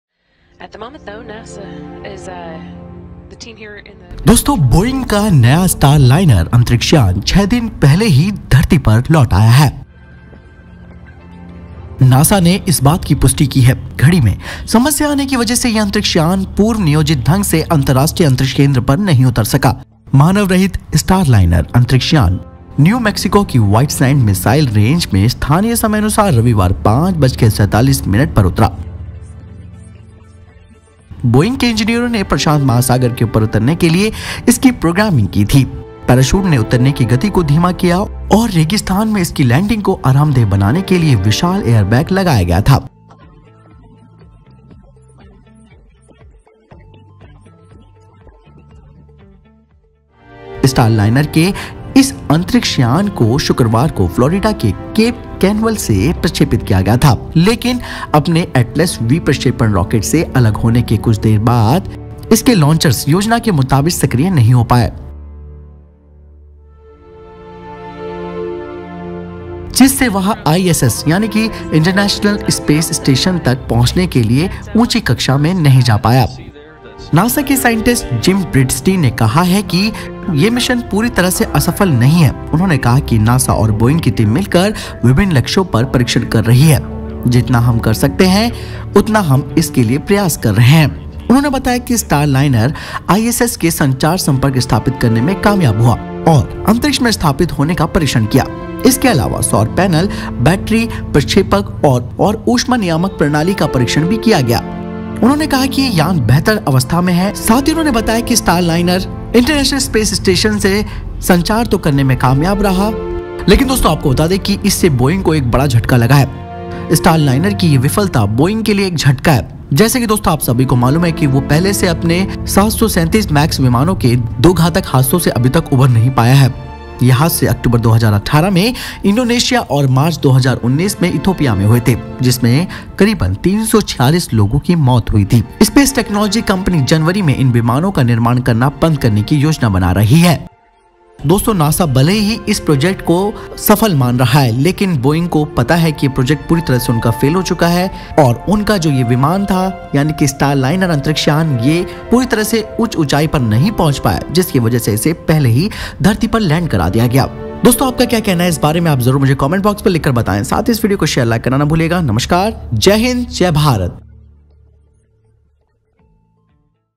नासा और बोइंग का मिशन हुआ फेल/ Boeing Starliner lands after mission failure
Boeing's new Starliner spacecraft will return to Earth on Sunday, six days early, after a clock problem prevented a rendezvous with the International Space Station, NASA and the aerospace giant confirmed Saturday.